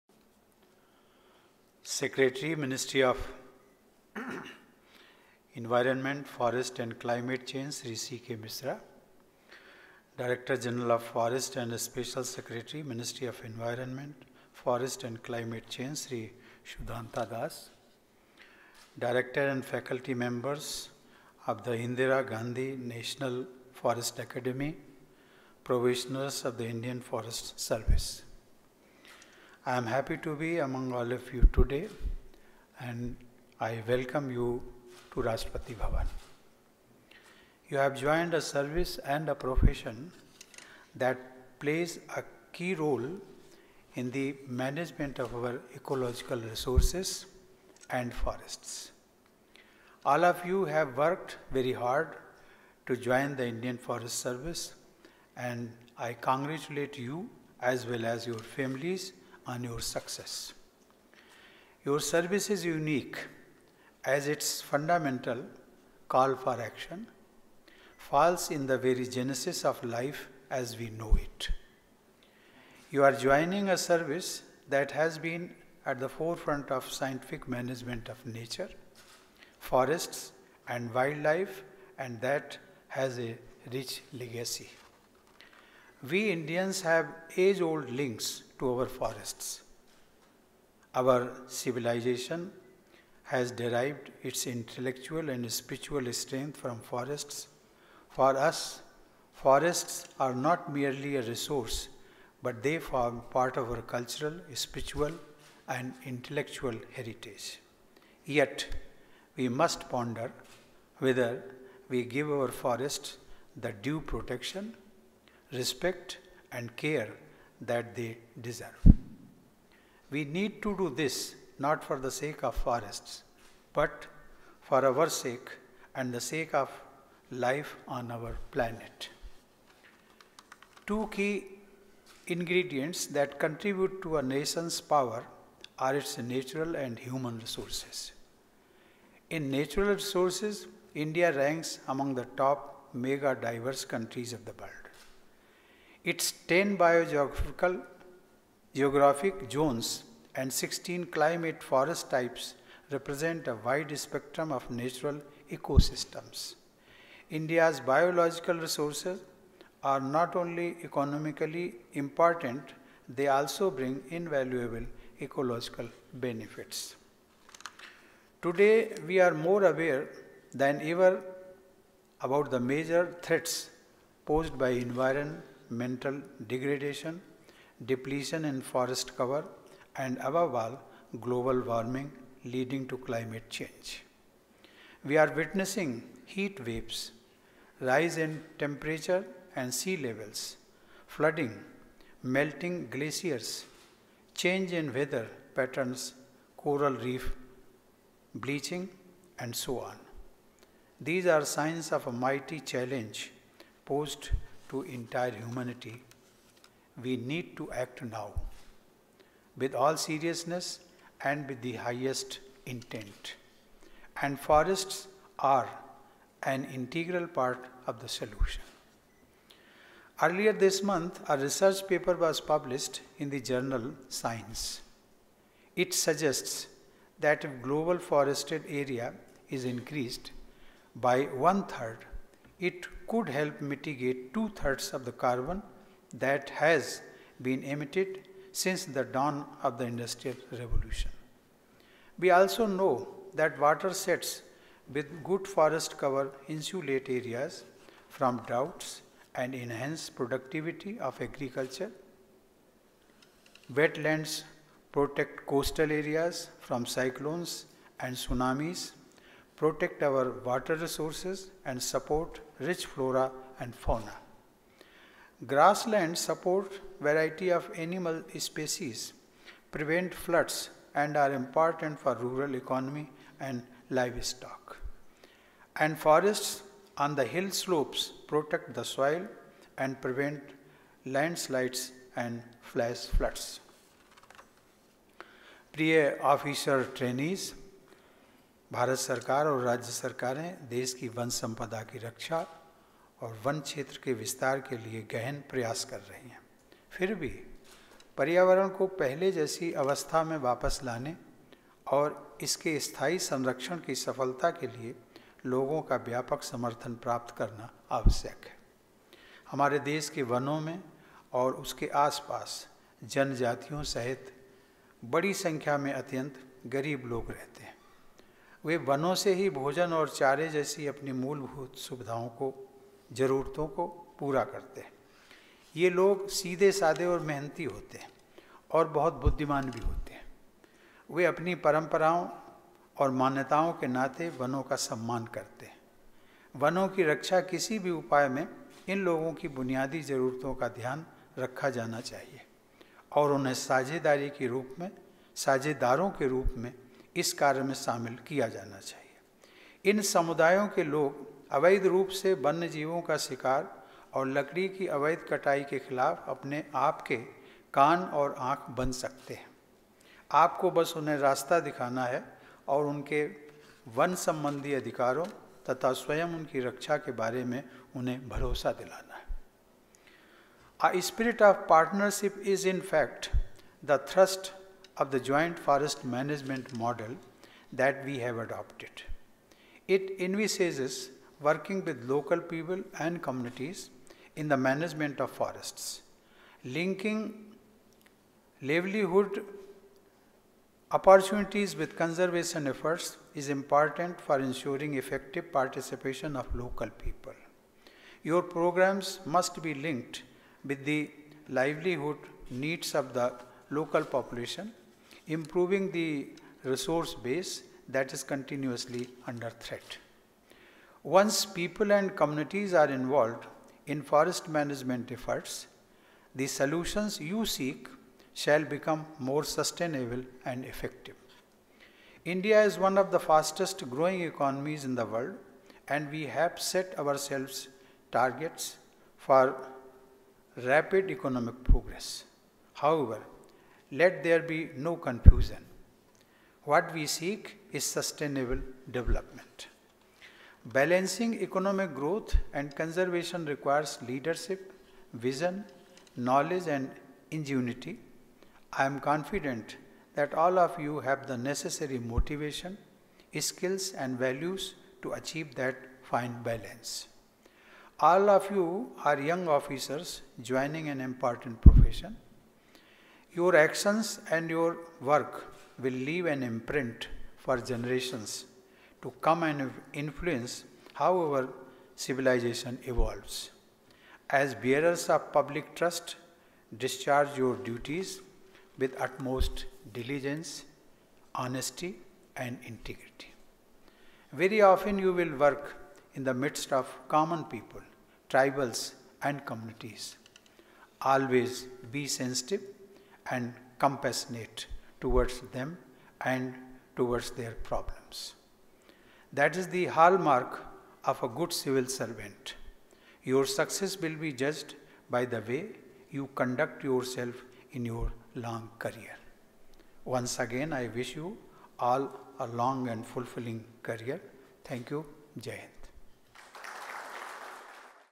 President Kovind meets probationers of the Indian Forest Service (2018-20 batch)
A group of probationers of the Indian Forest Service (2018-20 batch) called on the President of India, Shri Ram Nath Kovind, today (July 23, 2019) at Rashtrapati Bhavan.

Addressing the probationers, the President said that the Government of India and state governments are making considerable efforts to protect the forest wealth of our country and increase green cover. However, sustainability and success of ecological restoration and conservation lies in mass mobilisation. A large number of poor people, including tribals, live in and around the forests of our country. It is through forests that they fulfil their basic needs of food and fodder. These people are simple and hard-working - and they are very wise. They respect forests as part of their traditions and beliefs. Any measures to protect forests should be sensitive to the basic needs of these people and involve them as partners.

The President said thatthe joint forest management model that India has adopted envisages working with local people and communities in the management of forests. Linking livelihood opportunities with conservation efforts is important for ensuring effective participation of local people. The President said that once people and communities are involved in forest management efforts, the solutions the Forest Officers seek shall become more sustainable and effective.